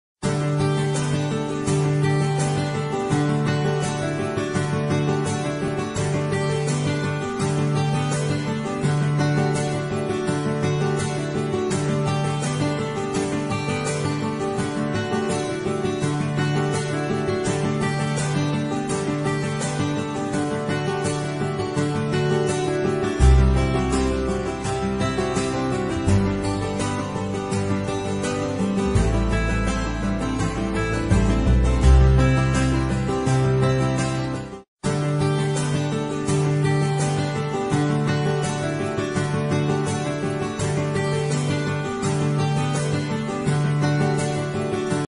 Church roof truss and sheet work.
Church roof structure and sheeting work. Tata hollow sections and Tata blue scope roofing sheets.